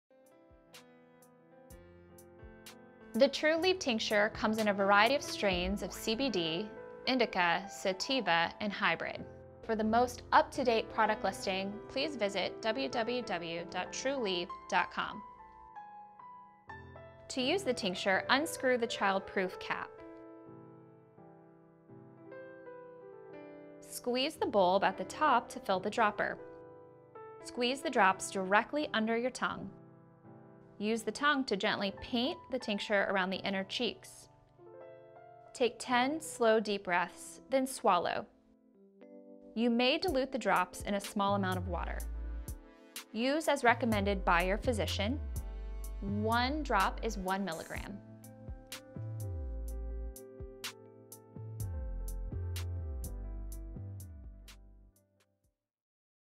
How to Use Trulieve's Oral Cannabis Oil Tincture
Trulieve offers a variety of products at its Florida medical marijuana treatment centers.  This is a video demonstrating how to use their cannabis oil tinctures.  Tinctures are a sublingual formulation designed to be taken orally under the tongue.  This provides better bio-avaiability by directly targeting the sublingual gland which is underneath the tongue.